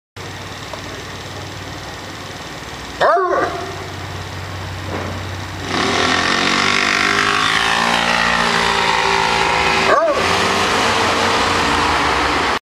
Big Barking Dog startled by moped
He was wondering where his owner was.